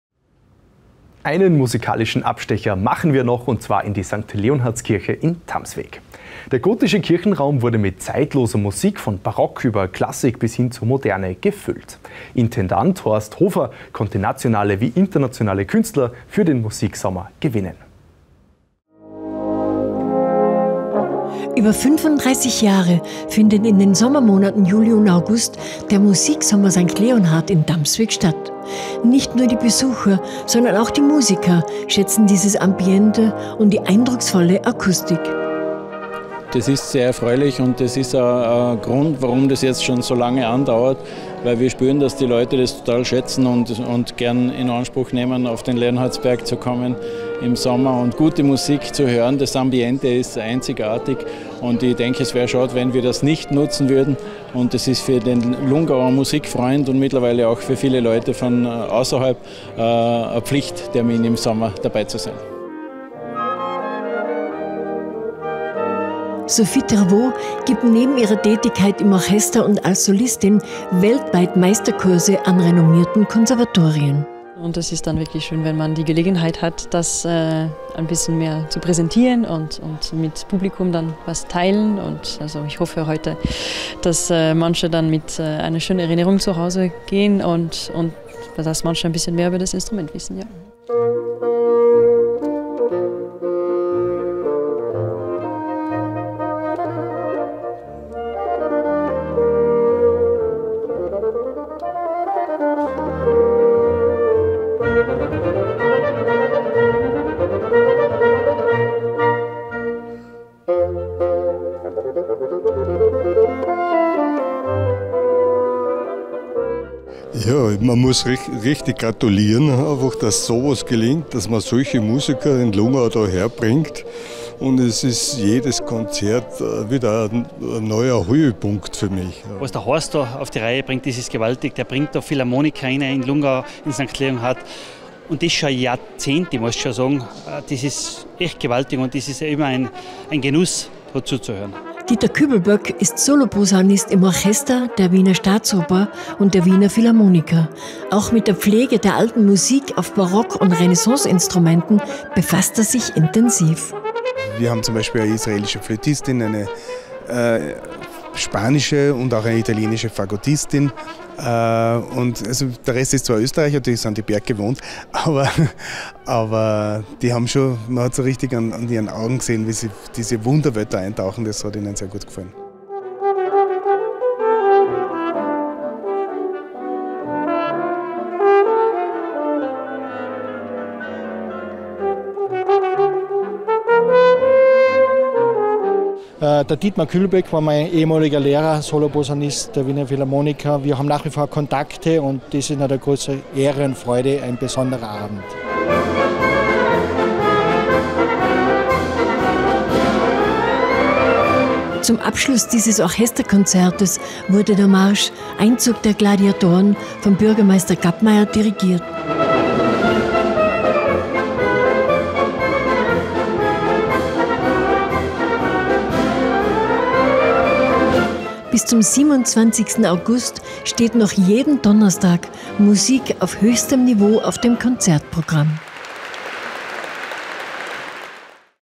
Musiksommer Sankt Leonhard 2022
Der gotische Kirchenraum in der Stankt Leonhardskirche in Tamsweg wurde mit zeitloser Musik von Barock, über Klassik bis hin zur Moderne gefüllt. Intendant Horst Hofer konnte nationale wie internationale Künstler für den Musiksommer gewinnen.